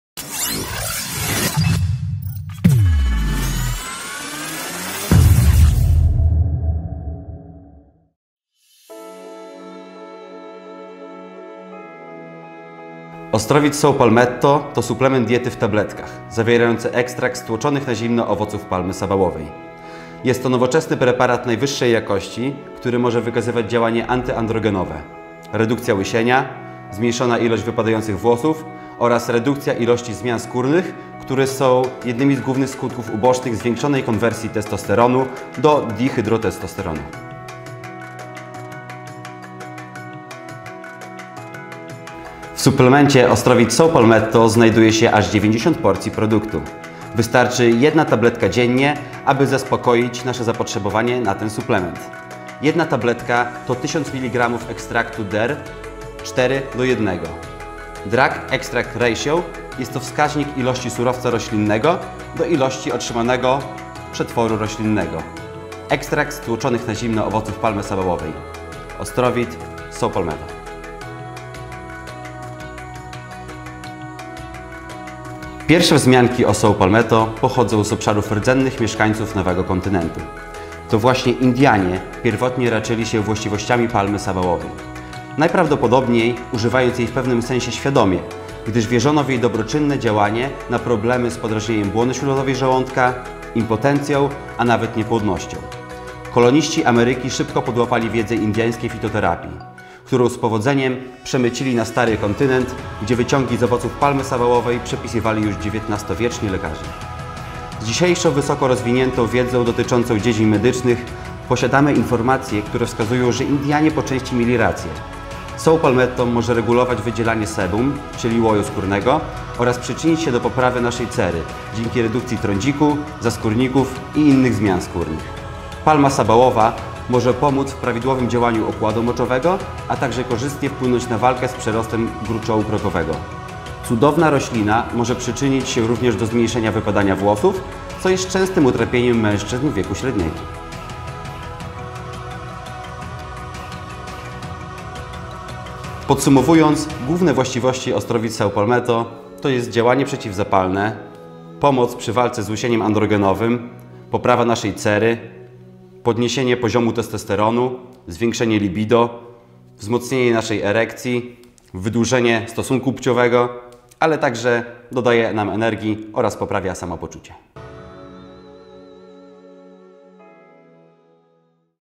Suplement OstroVit Saw Palmetto - działanie, dawkowanie, właściwości
Dzisiejszym suplementem jaki wzięliśmy pod lupę jest...

OstroVit Saw Palmetto w tabletkach, zawierający ekstrakt z tłoczonych na zimno owoców palmy sabałowej. Jest to nowoczesny i najwyższej jakości preparat, który może wykazywać działanie antyandrogenowe ( redukcja łysienia, zmniejszona ilość wypadających włosów oraz redukcji ilości zmian skórnych które są jednymi z głównych skutków ubocznych zwiększonej konwersji testosteronu do dihydrotestosteronu.)

Chcesz dowiedzieć się więcej o Saw Palmetto?